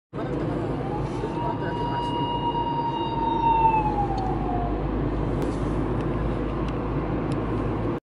愛知県警のパトカー緊急走行 愛知県日進市藤塚（国道153号線）付近 2021.1.3
愛知県日進市藤塚（国道153号線）付近を豊田市方面に走行中、反対車線から愛知県警のパトカーが緊急走行していました。